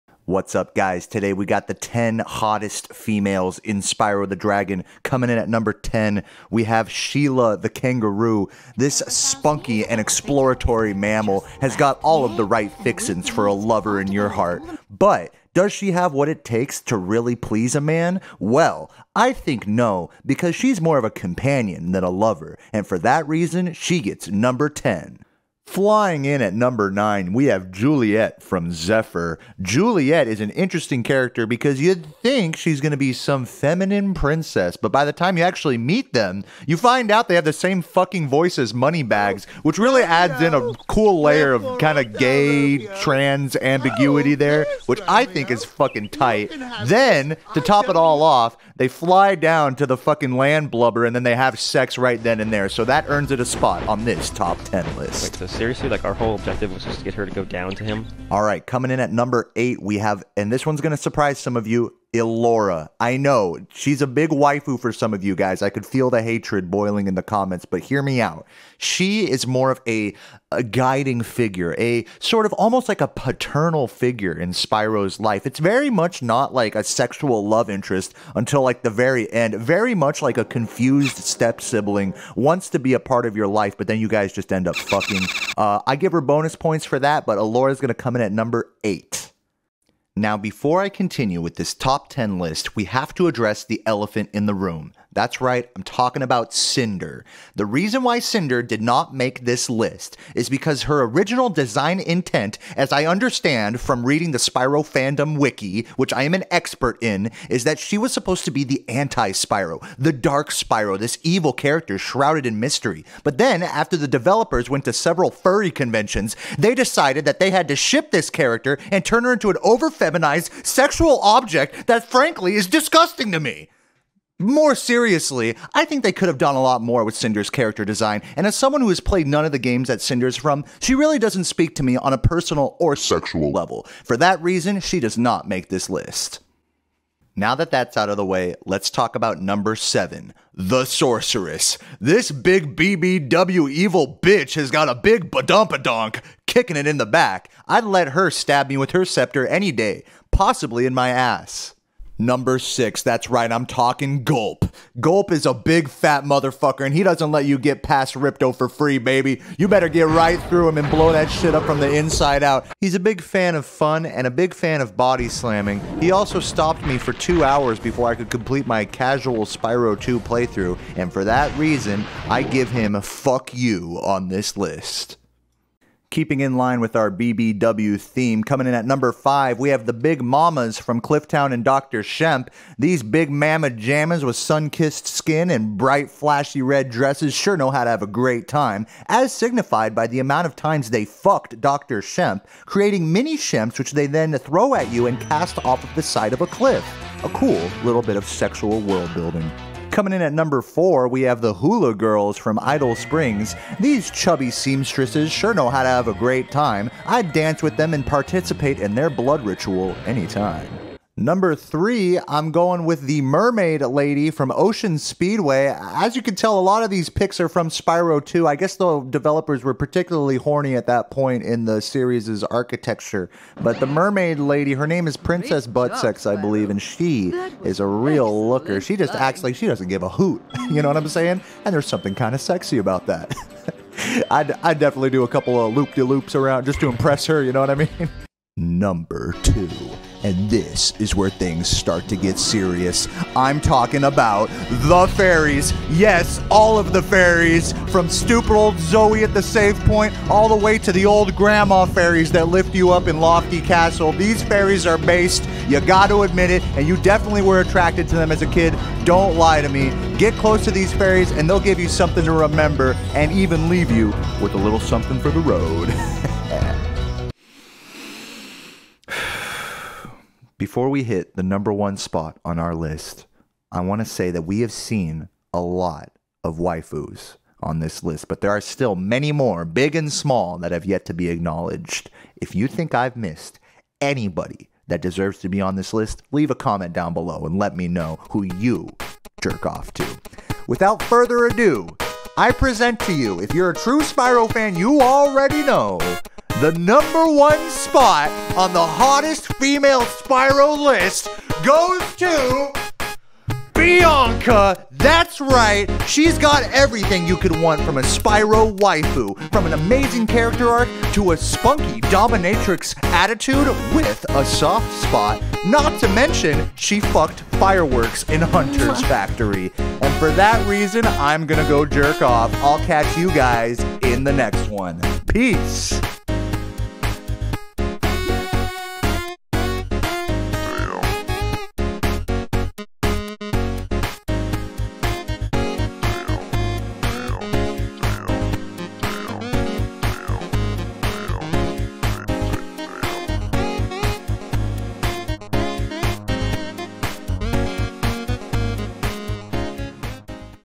Top 10 HOTTEST Spyro Women
These gals are really smokin

LEWDacris8008 - Cropped fairy booty (its on newgrounds, can't link because porn)
I've blurred out art from the cinder section due to a request from one of it's creators.

(banger alert)